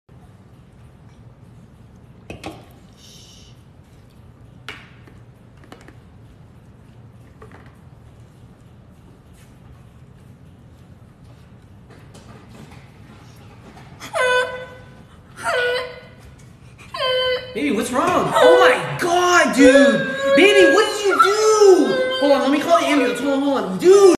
Tongue Prank On Boyfriend | Amani & Israel
he really called 911😳😂